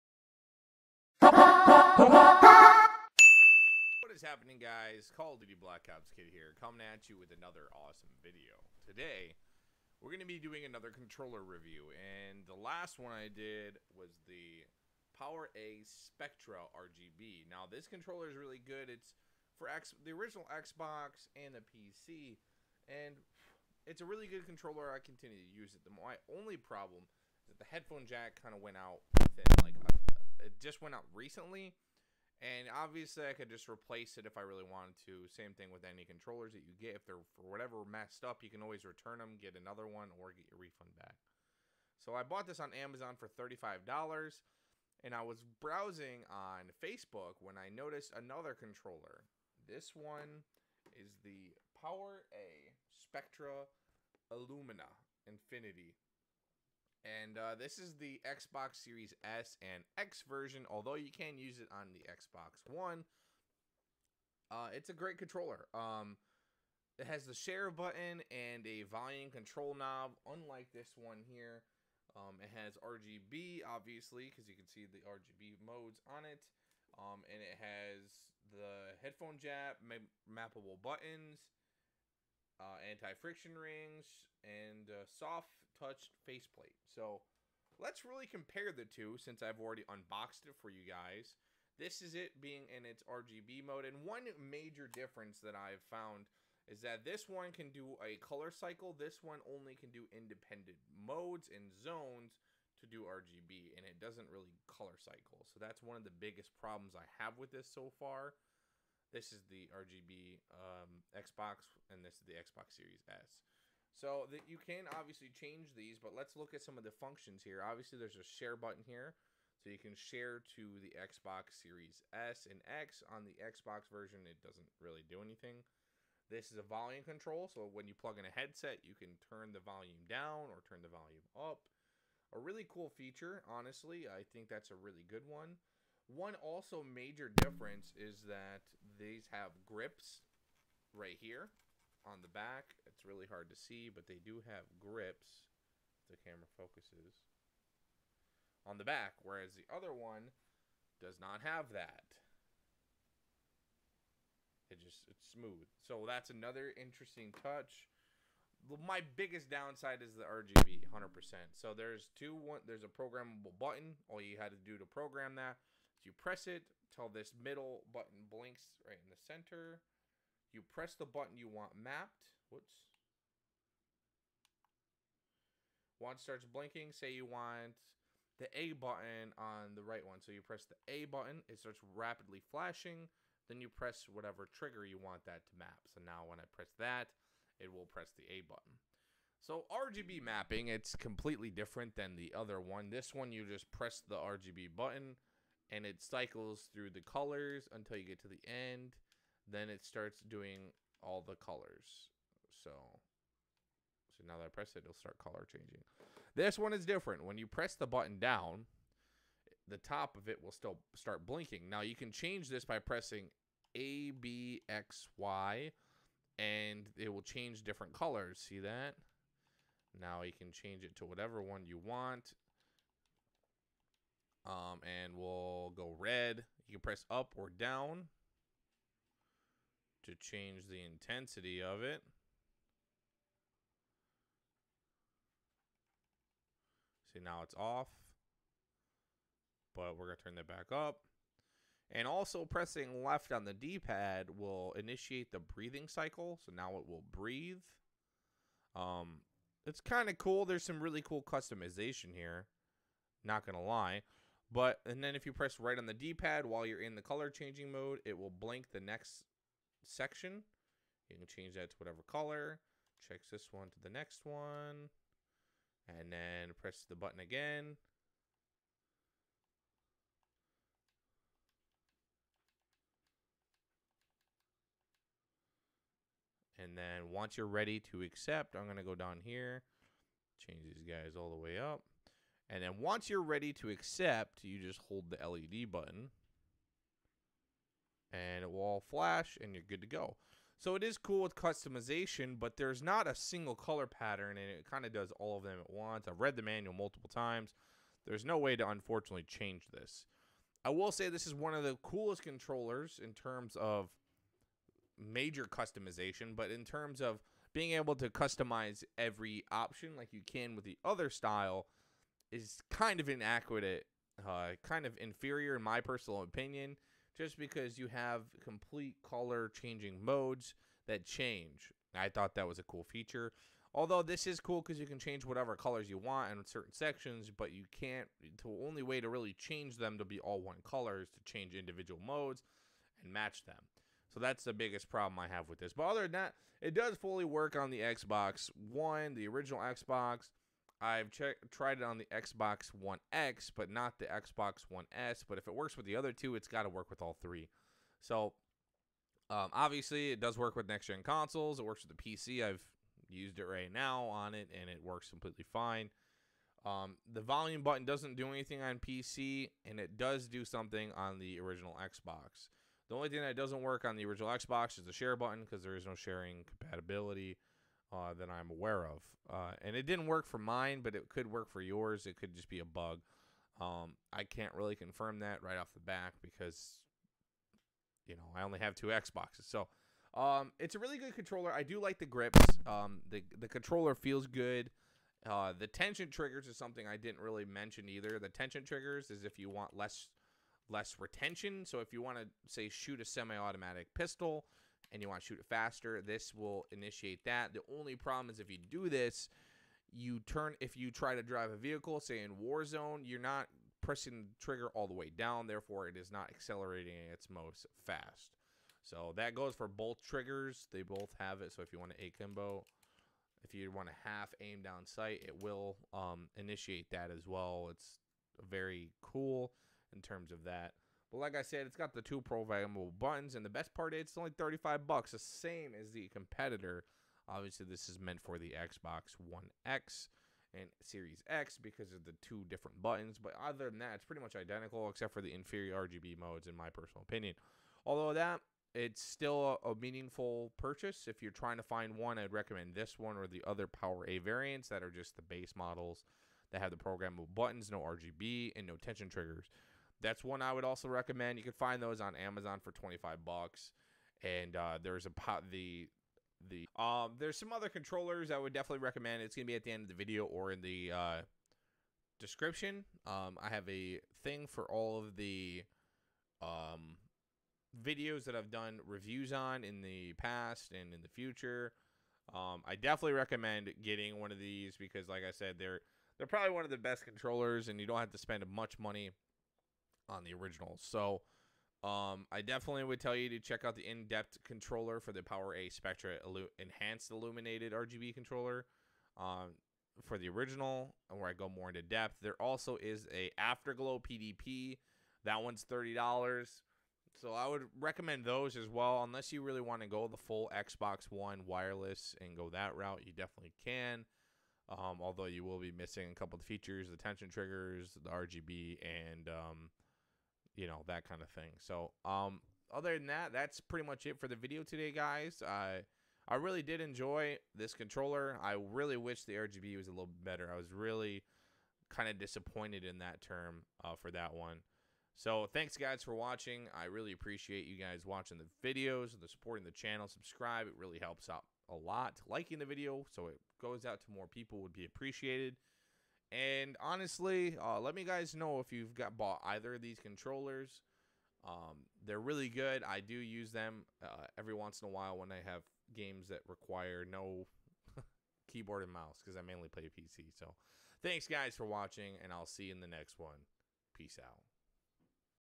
The Best Next Gen Controller EVER!!! PowerA Spectra Infinity Enhanced Wired Controller Review!!
This is the best controller ever for next gen xbox and old gen xbox, with 2 programable buttons, tension triggers, and RGB!!